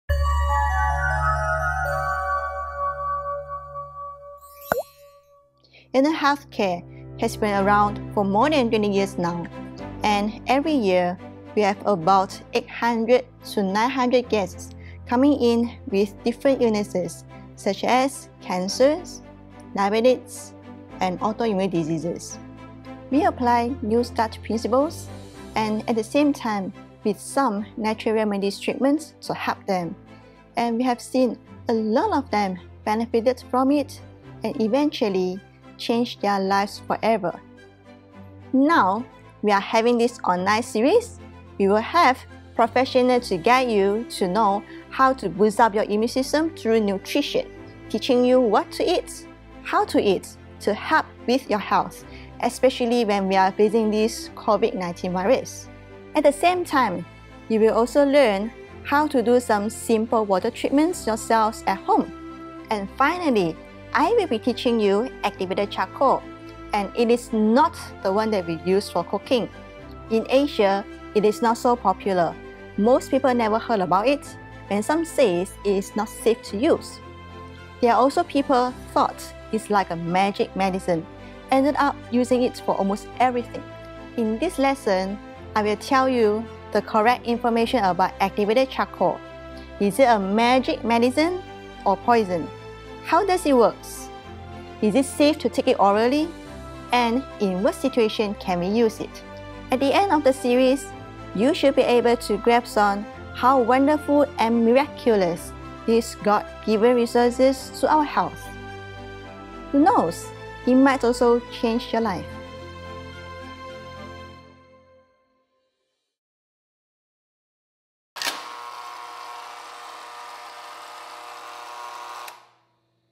Simple Home Remedies Lecture Trailer
Teaching what are the nutrition that you can take to boost your immune system (Part 1&2)
Hydrotherapy Part 1&2 (Water treatments that you can do at home for treating certain long term illnesses through boosting your immune system)
Activated Charcoal (How to use it in a proper way)